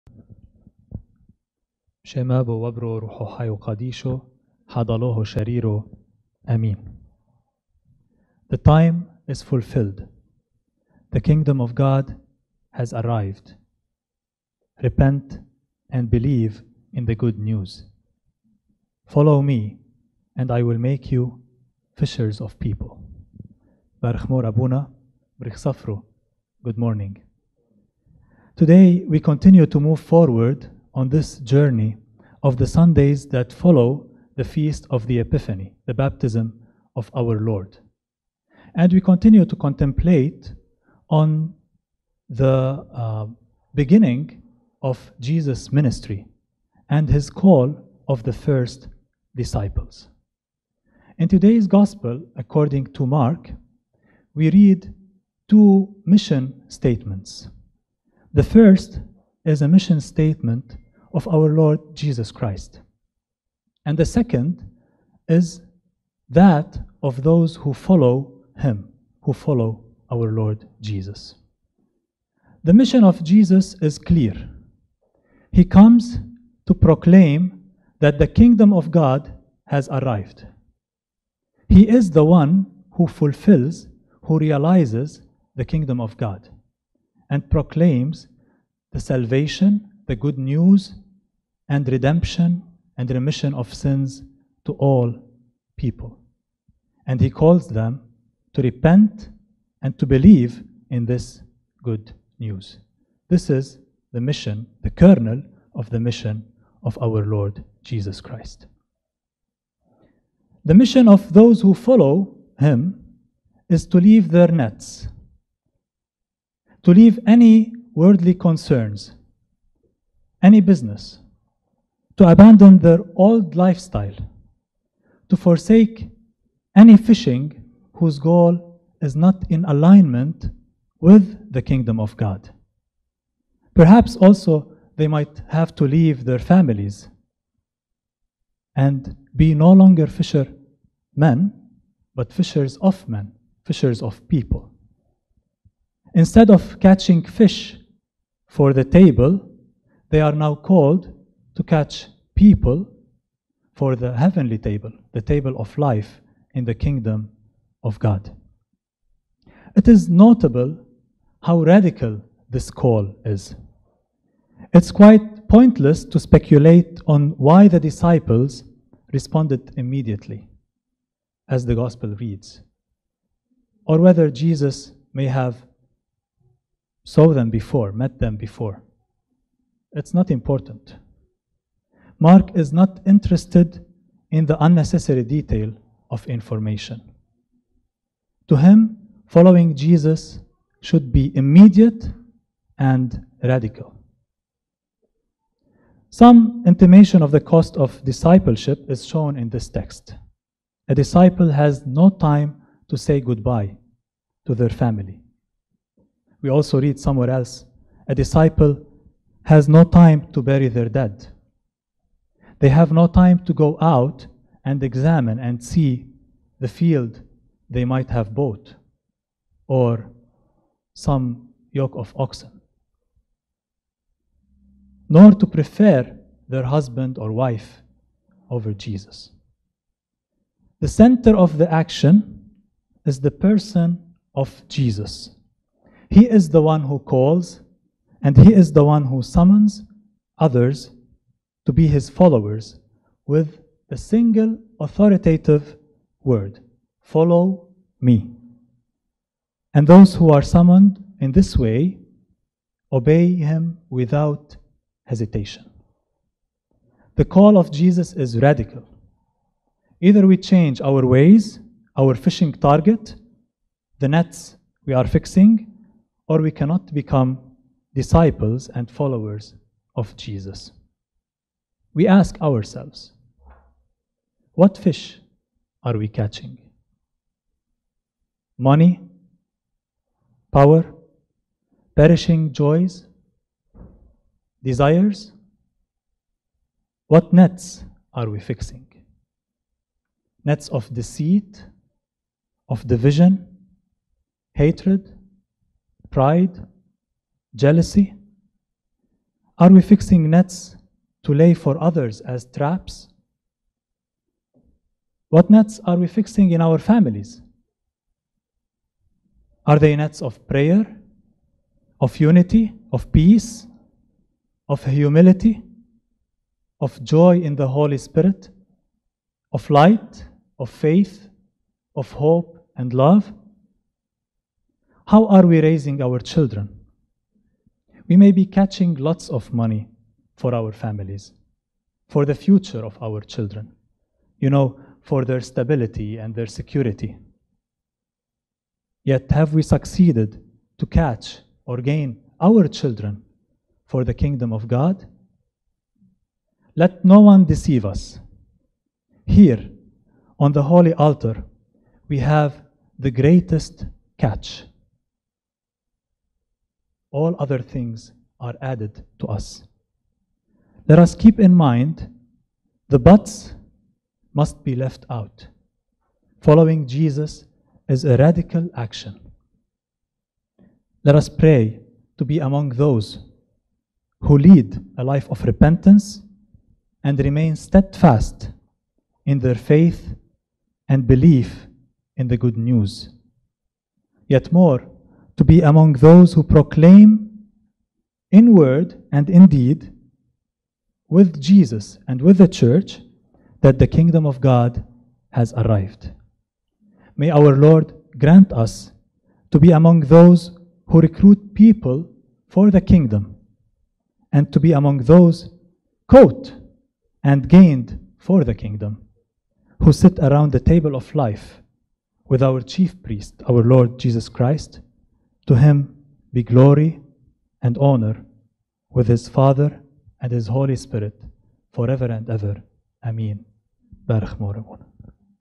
Becoming "Fishers of Men" - Assyrian Orthodox Church of the Virgin Mary: February 14, 2021
Sermon from Shamosho Imad Syryany from our church in Paramus, NJ.

Today, we continue to move forward on this journey of the Sundays that follow the feast of the Epiphany, the baptism of our Lord. And, we continue to contemplate on the beginning of Jesus’ ministry, and his call of the first disciples. In Today’s Gospel according to St. Mark, we read two mission statements. The first one is the mission statement of Jesus, while the second is that of those who follow Him.

The mission of Jesus is clear: He is the One who proclaims that the time is fulfilled, and the kingdom of God has arrived. He is the One who realizes this Kingdom. He brings salvation and remission of sins and grants eternal life to those who repent and believe in this good news.

The mission of those who follow Him is to leave their nets, to leave any worldly concerns and business, to abandon the old way of life, to forsake any fishing whose goal is not in alignment with the kingdom of God. Perhaps, they also leave their families to follow Jesus and become no longer fishermen but fishers of men. Fishers who fish people for the kingdom of God. Instead of catching fish for the table, they would recruit people for the table of life in the kingdom.

It is notable how radical this call is. It is quite pointless to speculate on why the disciples responded immediately, as the gospel reads, or whether Jesus had met them beforehand. Mark is not interested in the unnecessary detail of information. To him, following Jesus should be immediate and radical: The ‘but’s’ must be left out. Some intimation of the cost of discipleship is shown: A disciple has no time to say goodbye to their family, nor to bury their dead (cf. Mt 8:21-22, Lk 9:57-62). They have no time to go out and see and examine a field or some yoke of oxen they may have bought, nor to prefer their wife or husband (cf. Lk 14:16-24). The center of the action is the person of Jesus: "He is the one who calls and summons others to be His followers with the single authoritative word ‘Follow me!’; and those who are summoned in this way obey him without any hesitation."

The call of Jesus is radical: Either we change our ways, our fishing target, the nets we are fixing, or we cannot become disciples and followers of Jesus. We ask ourselves: What fish are we catching? Money? Power? Perishing joys? Desires? What nets are we fixing? Nets of deceit, division, hatred, pride? Are we fixing nets to lay for others as traps? What nets are we fixing in our families? Are they nets of prayer? Of unity? Of peace? Of humility? Of joy in the Holy Spirit? Of light? Of FAITH? Of HOPE? Of LOVE? How are we raising our children? We may be CATCHING lots of money for our families, for the future of our children, for their “stability” and “security;” yet, have we succeeded to catch/gain our children for the kingdom of god?

Let no one deceive us. HERE, ON THE HOLY ALTAR, IS OUR GREATEST CATCH; all other things are added to us (cf. Mt 6: 33). Let us keep in mind: The ‘but’s’ must be left out. Following Jesus is a radical action.

Let us pray to be among those who lead a life of repentance and remain steadfast in their faith and belief in the good news. Yet more, to be among those who proclaim in word and in deed, with Jesus and the Church, that the kingdom of God has arrived. May our Lord grant us to be among those who recruit people for the kingdom, and to be among those caught and gained for the kingdom, who sit around the table of life with our Chief Priest, our Lord Jesus Christ, to Him be glory and honor, with His Father and Holy Spirit, forever and ever, amen.